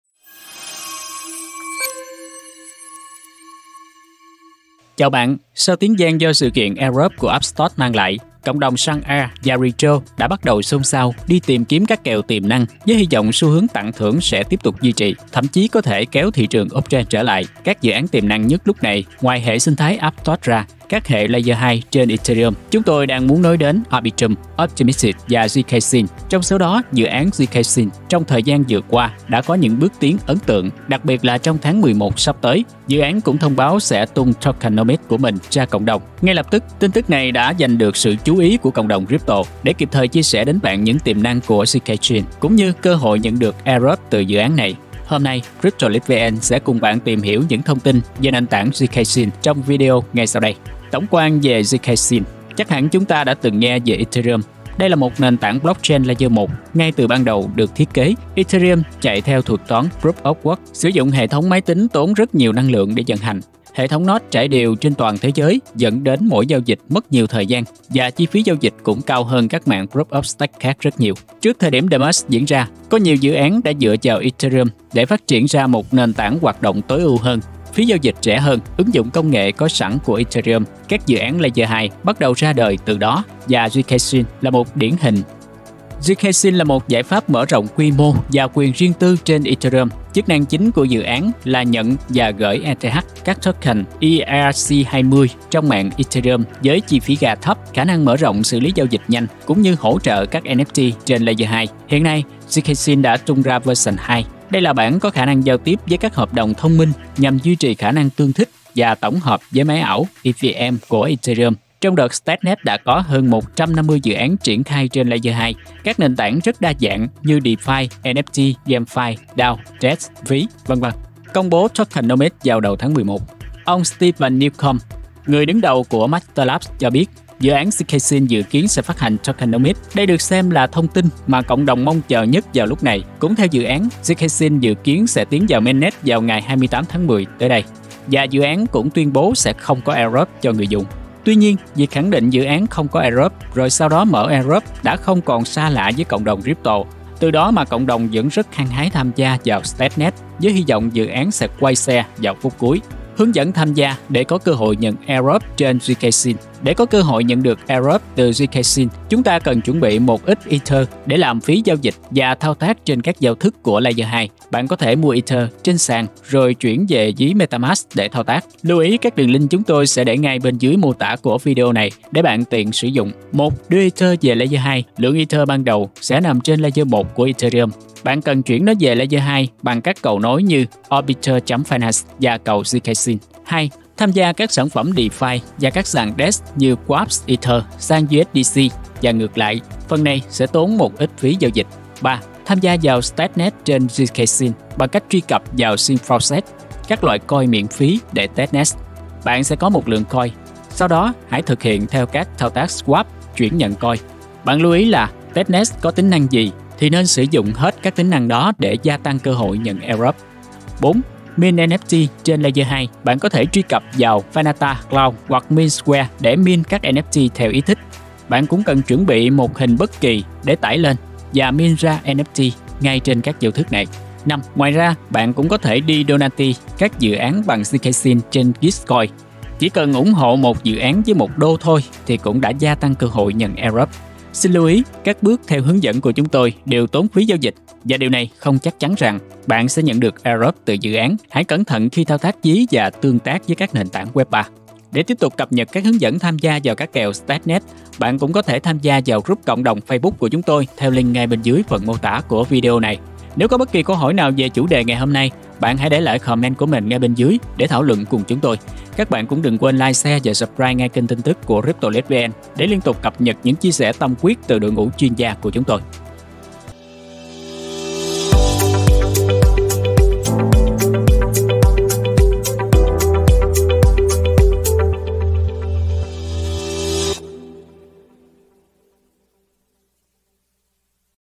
Hướng Dẫn Nhận Airdrop Trên Zksync
Chào Bạn, sau tiếng vang do sự kiện Airdrop của Aptos mang lại, cộng đồng săn Air và Retro đã bắt đầu xôn xao đi tìm kiếm các kèo tiềm năng với hi vọng xu hướng tặng thưởng sẽ tiếp tục duy trì. Thậm chí có thể kéo thị trường uptrend trở lại.

Các dự án tiềm năng nhất lúc này, ngoài hệ sinh thái Aptos ra, là các hệ Layer 2 trên Ethereum. Chúng tôi đang muốn nói đến Arbitrum, Optimistic và zkSync. Trong số đó, dự án zkSync trong thời gian vừa qua đã có những bước tiến ấn tượng. Đặc biệt là trong tháng 11 sắp tới, dự án cũng thông báo sẽ tung “tokenomic” của mình ra cộng đồng.

Ngay lập tức tin tức này đã dành được sự chú ý của cộng đồng Crypto. Để kịp thời chia sẻ đến bạn những tiềm năng của zkSync, cũng như cơ hội nhận Airdrop từ dự án này. Hôm nay, CryptoLeakVN sẽ cùng Bạn tìm hiểu những thông tin về nền tảng zkSync trong video ngay sau đây